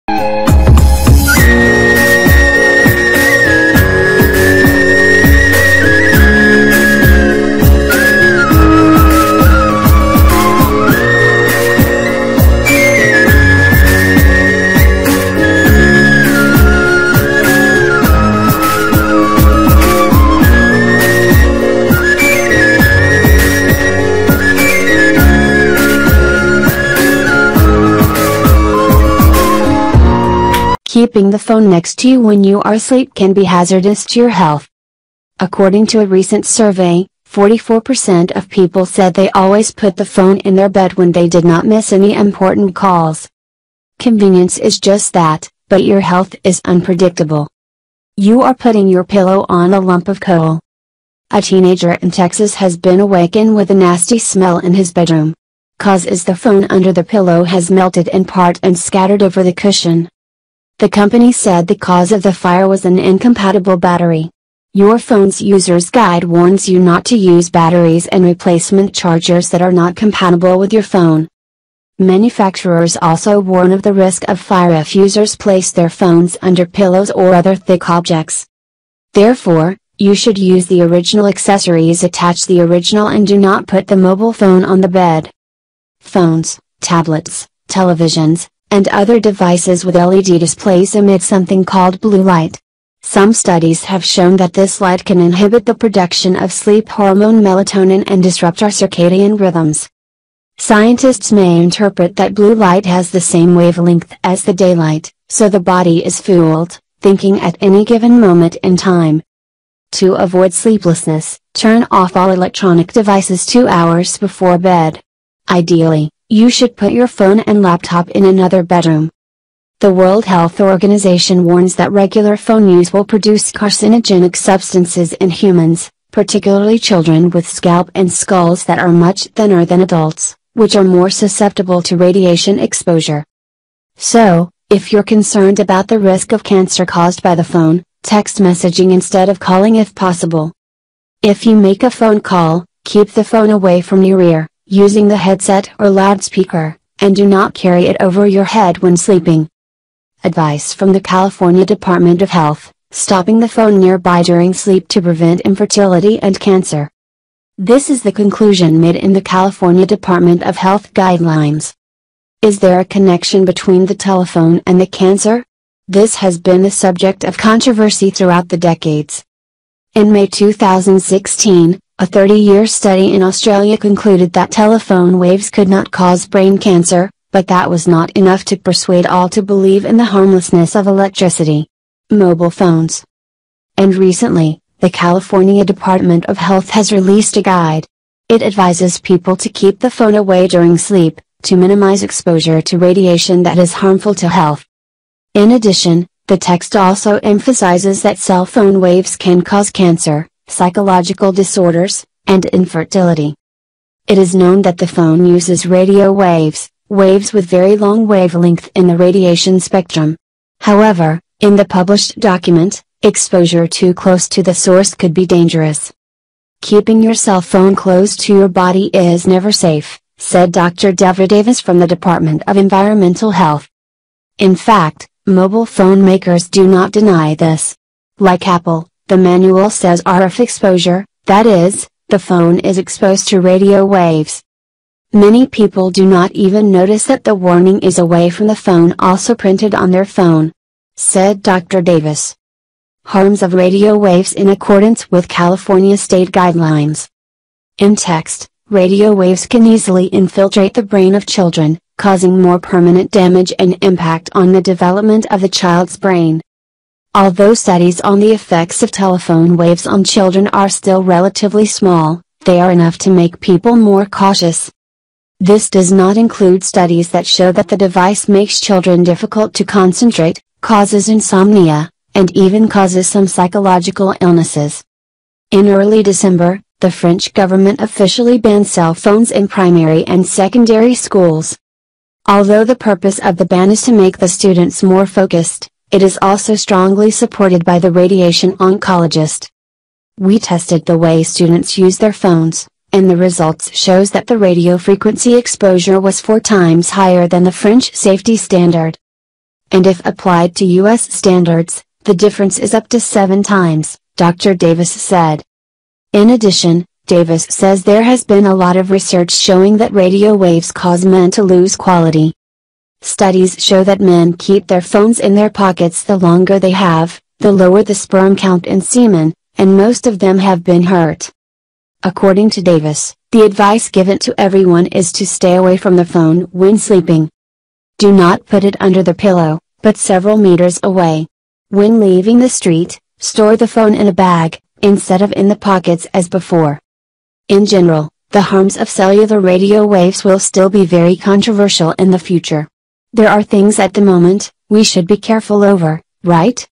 The habit of getting the phone close while sleeping - After a long time this will happen to you
-Health Knowledge The habit of getting the phone close while sleeping  After a long time this will happen to you